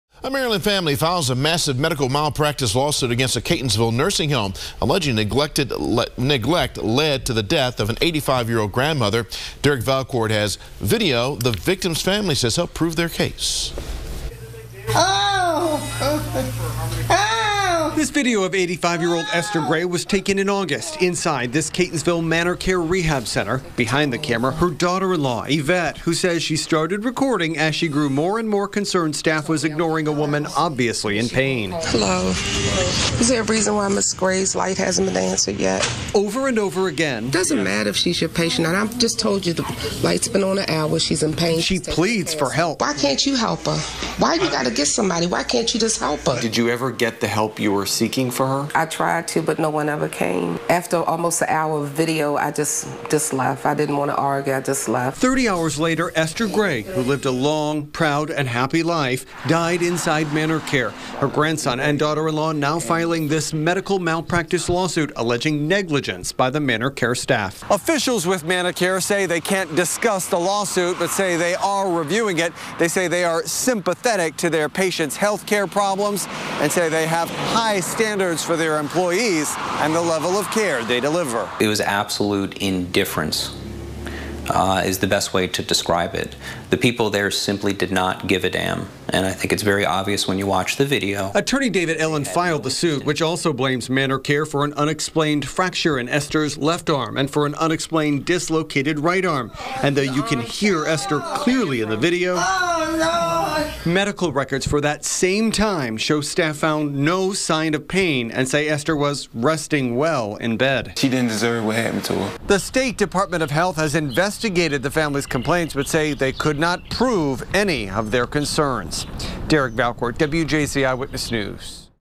Family Alleges Negligence At ManorCare In Catonsville
The family of an 85-year-old woman who died last year has sued a Catonsville nursing home, saying video shot by her daughter-in-law shows that the staff ignored her cries of pain for more than an hour.